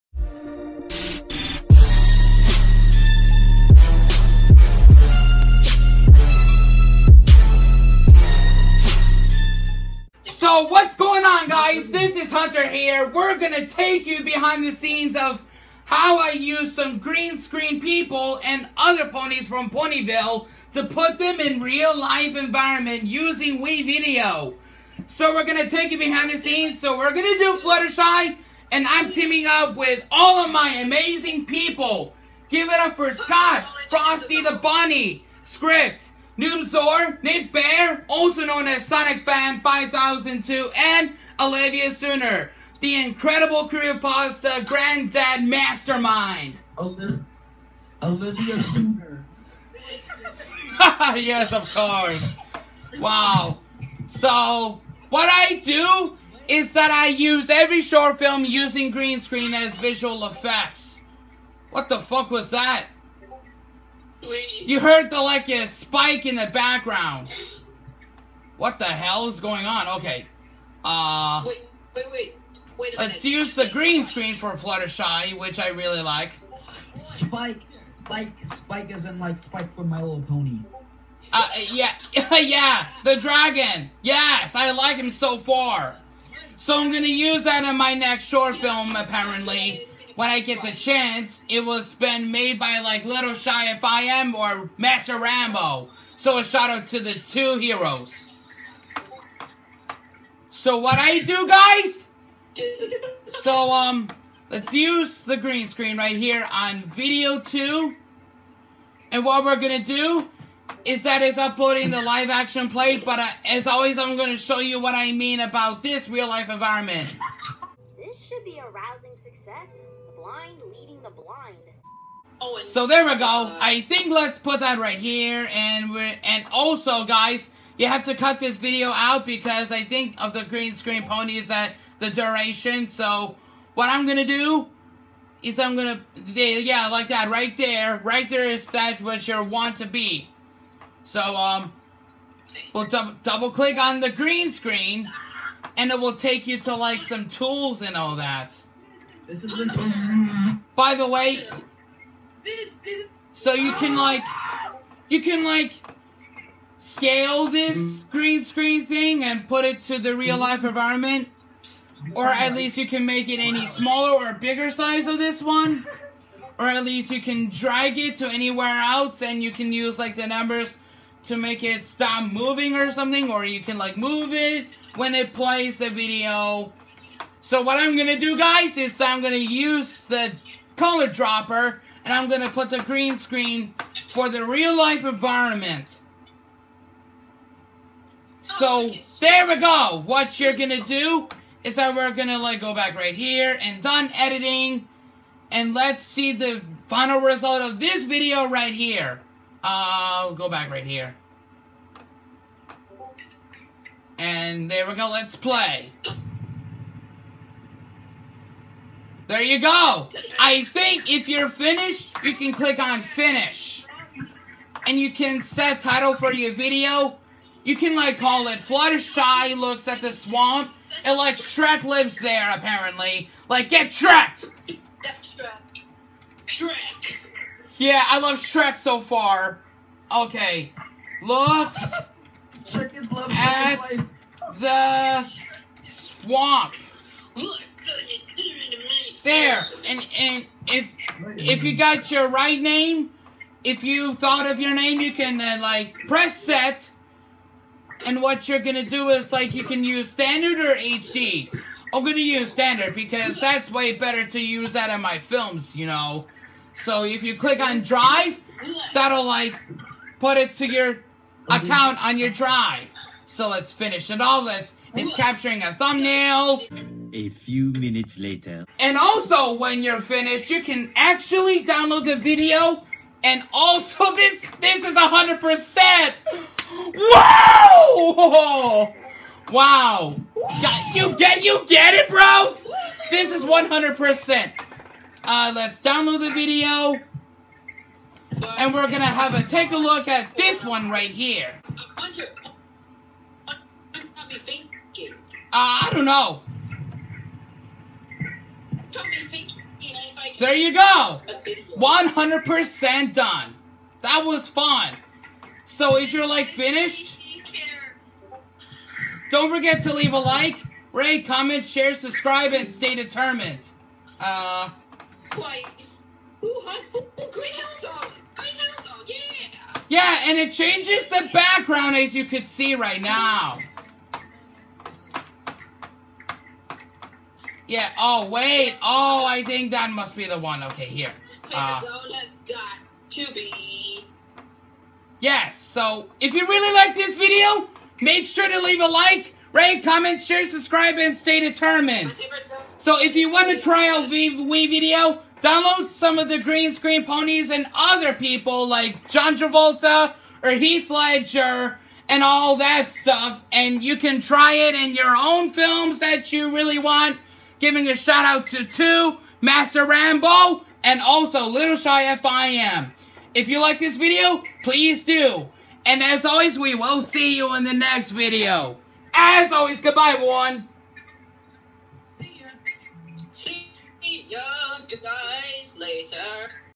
From Green Screen To Real Life Environment BTS (How I Make WeVideo VFX) [W/ Friends]
We are going to take you behind the scenes of how I make green screen and put them in real life since I made several of them in my films which you all enjoyed so far, and I hope you enjoy this video guys!